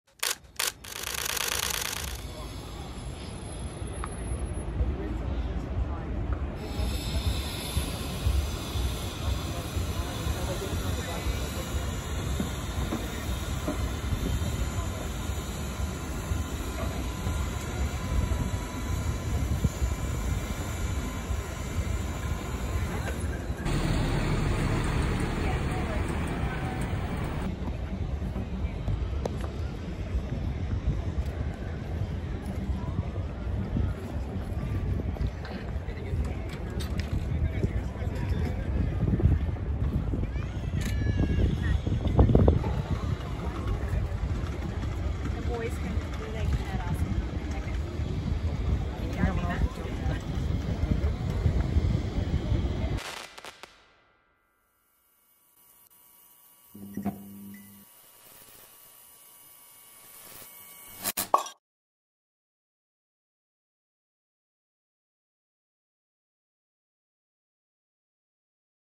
Rebel Wilson Arrives To The AACTA On Golf Cart
Rebel Wilson is seen arriving at the Sydney Opera House to attend the AACTA Awards with her good friend, podcaster and former Bachelor runner-up Brittany Hockley on a golf cart.

This video was taken 08 Dec 2021 by K Thatcher / MEGA